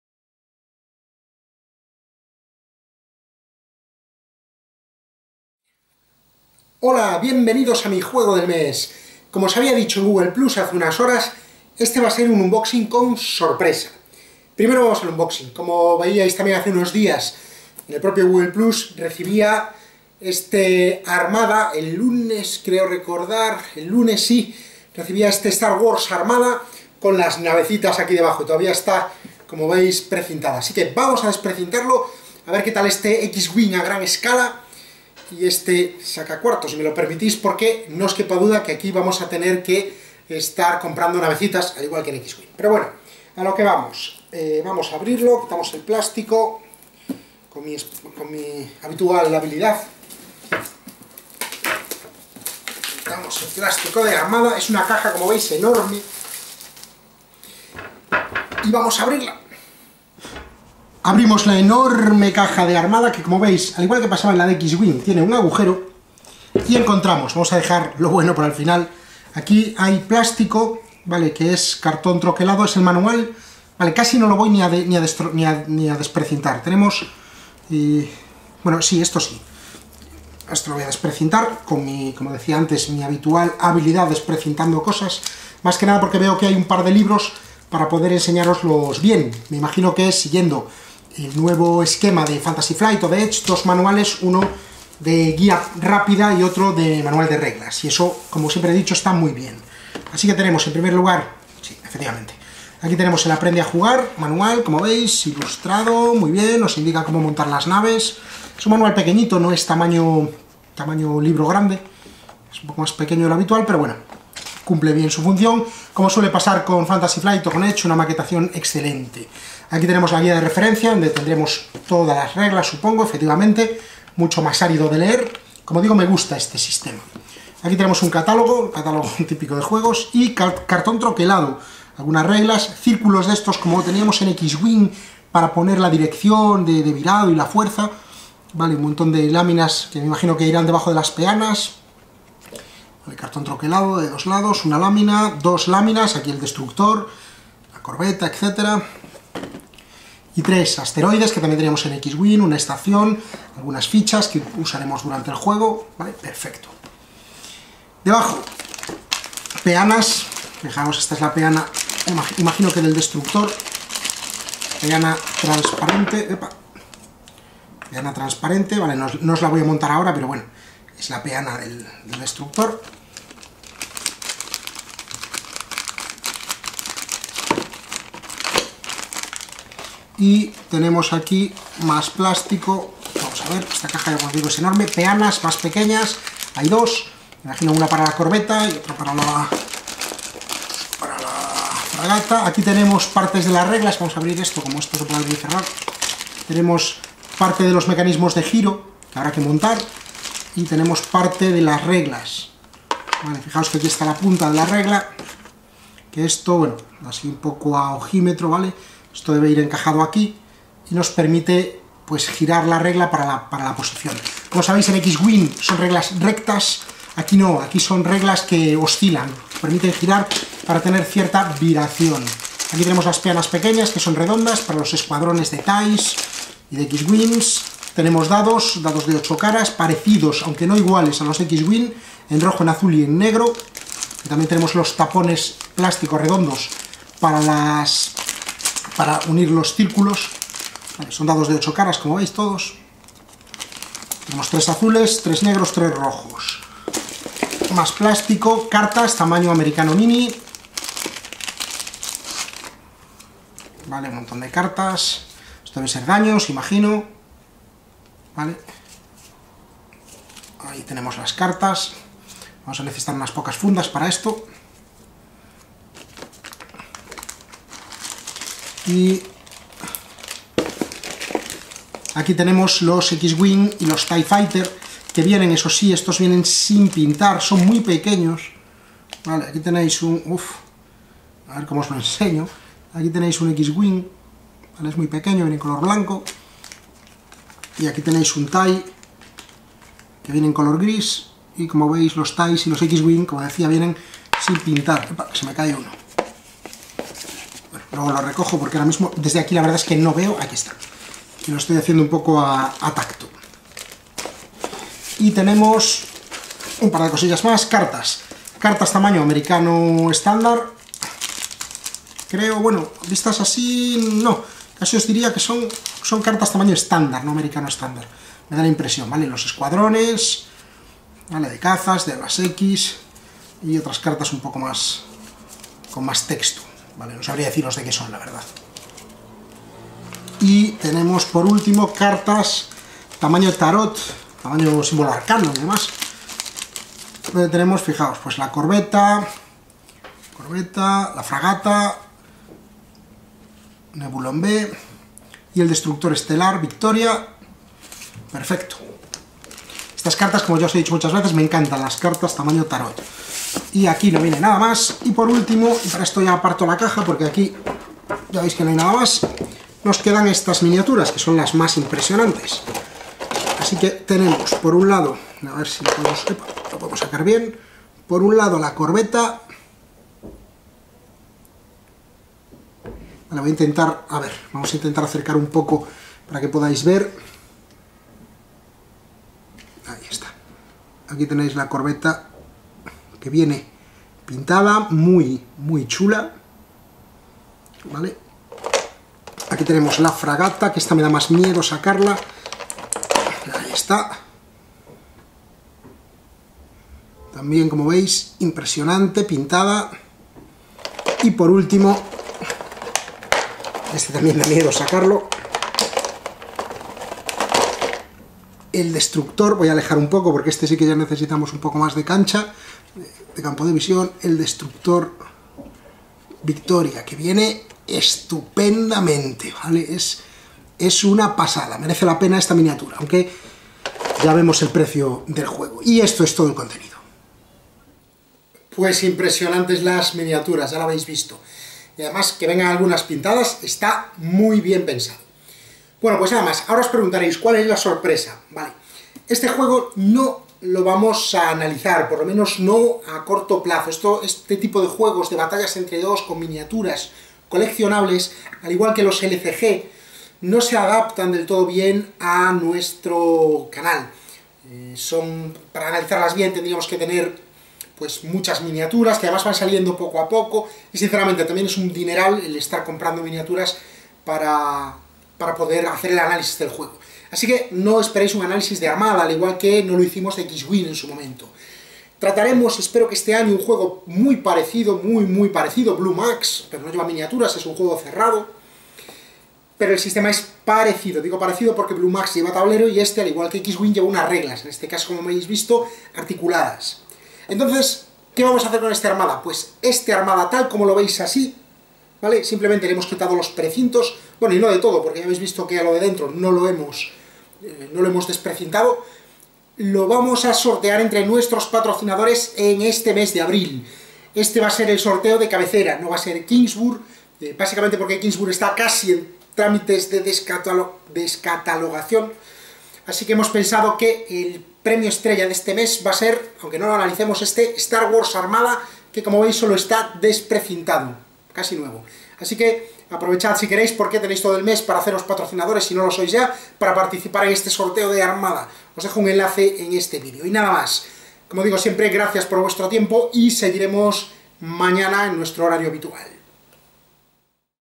MJDM - Unboxing Star Wars Armada
Abrimos la caja de Armada, de Star Wars.